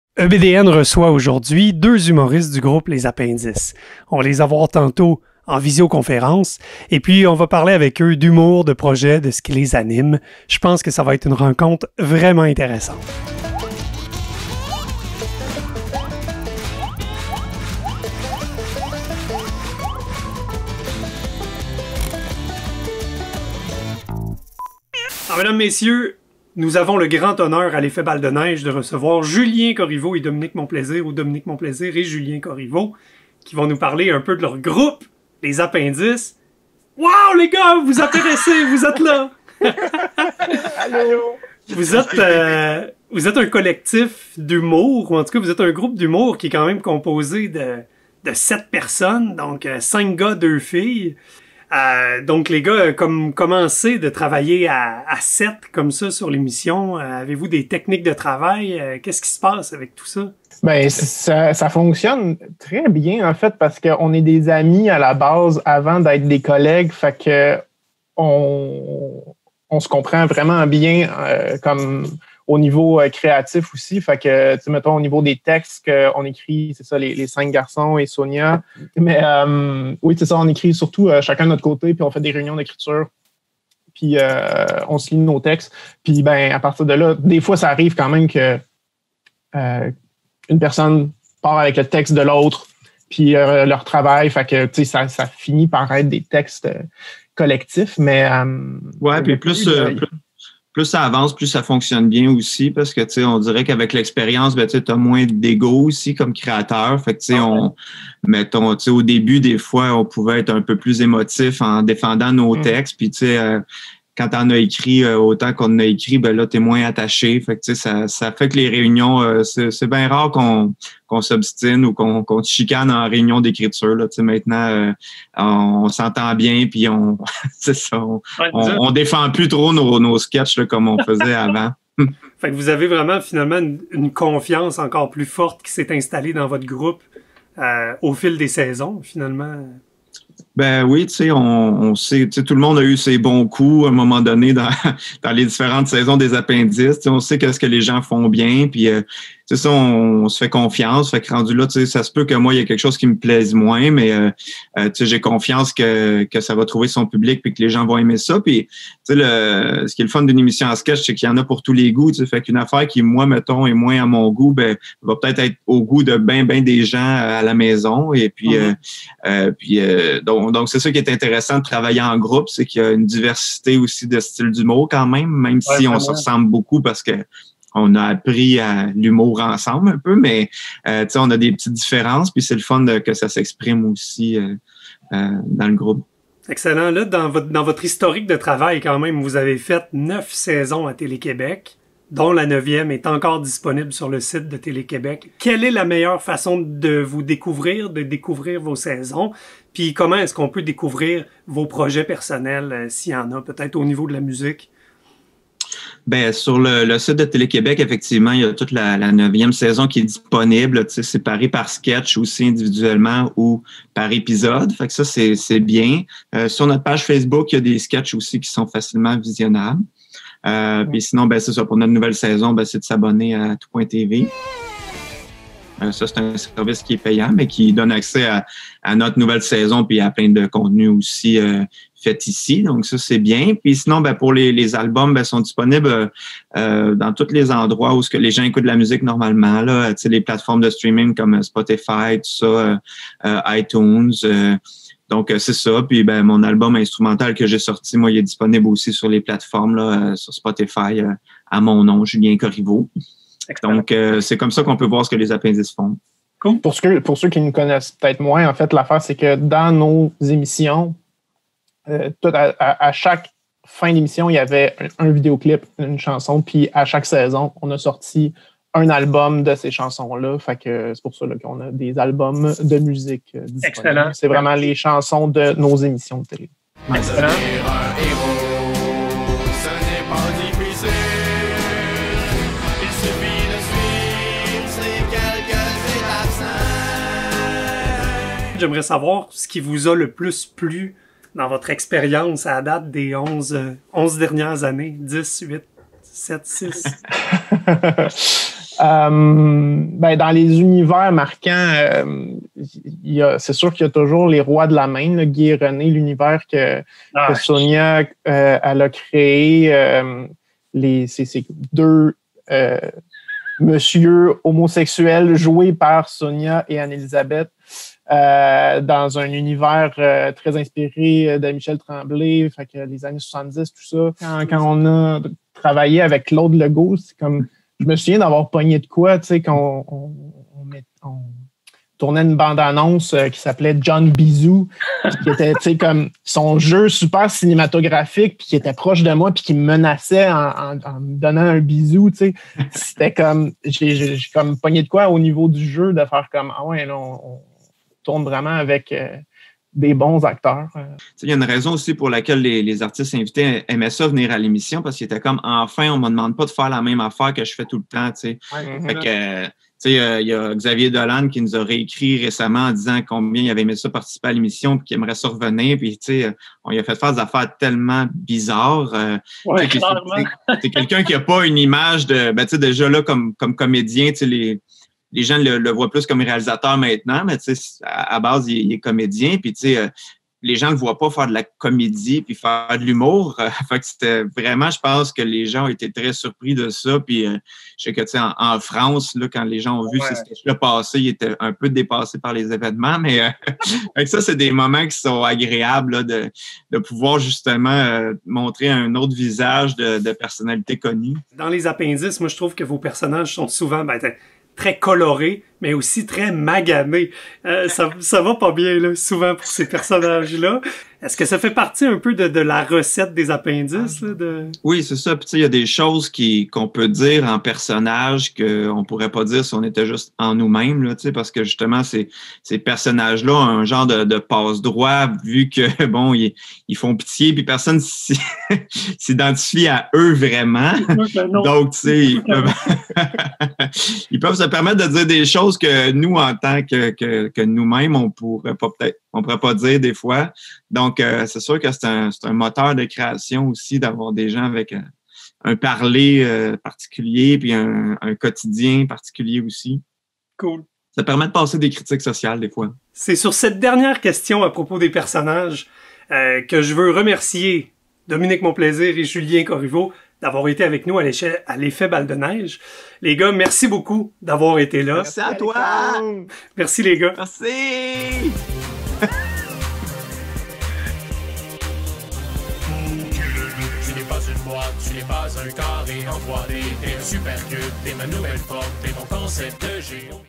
Découvrir l'humour du Québec avec Les Appendices
Nous croyons que cette rencontre avec deux membres des Appendices vous permettra de faire découvrir leur riche et généreux univers humoristique à vos étudiants.

Pour connaitre d'autres artistes de l'humour québécois, Julien et Dominic vous proposent de découvrir :
- Bruno Blanchet
- Marc Labrèche
- Les Denis Drolet
- Les Chick n' swell
- Rock et belles oreilles
- Les Bleu poudre
- François Perrusse

-EBDN remercie le cégep du Vieux-Montréal pour le prêt de matériel qui rend possible ces vidéos.